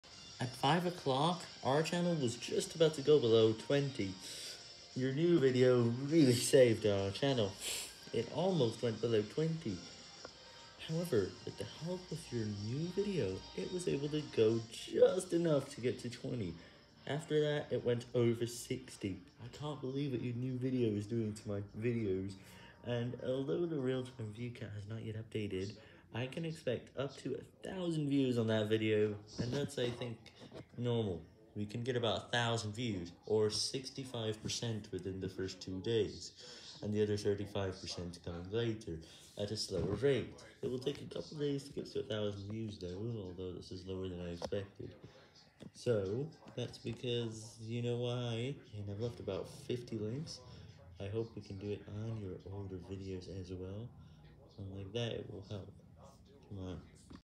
At 5:00, my channel nearly went below 20.
But then, the new video was just enough to get the number to 20.